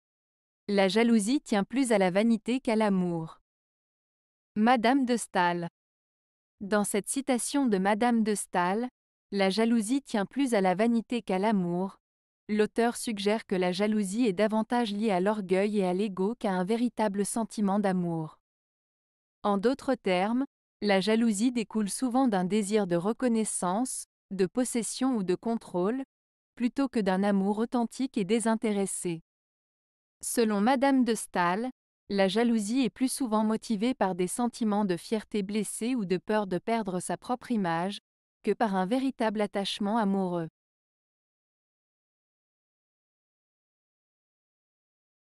Mme de Staël - La jalousie tient plus à la vanité qu'à l'amour.
Dans cette citation de Mme de Staël, "La jalousie tient plus à la vanité qu'à l'amour", l'auteure suggère que la jalousie est davantage liée à l'orgueil et à l'ego qu'à un véritable sentiment d'amour. En d'autres termes, la jalousie découle souvent d'un désir de reconnaissance, de possession ou de contrôle, plutôt que d'un amour authentique et désintéressé. Selon Mme de Staël, la jalousie est plus souvent motivée par des sentiments de fierté blessée ou de peur de perdre sa propre image, que par un véritable attachement amoureux.